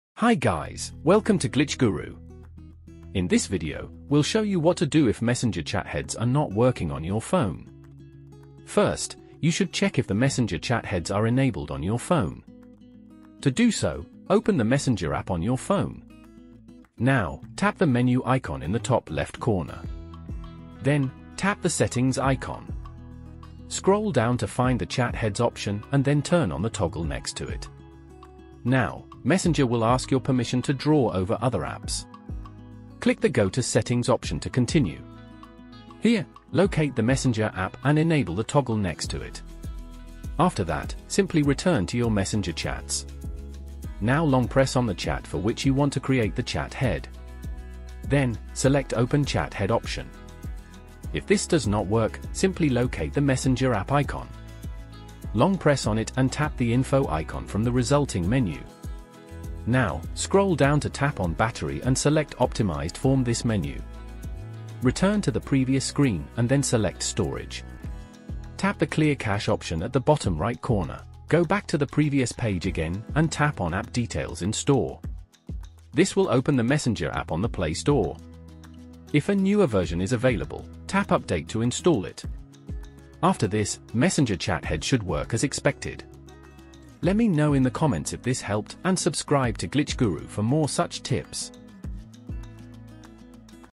[FIXED] Messenger Chat Heads Not Working on Android (2024)
If Facebook Messenger chat heads are not showing or working on your Android phone, this video will show you how to fix that quickly.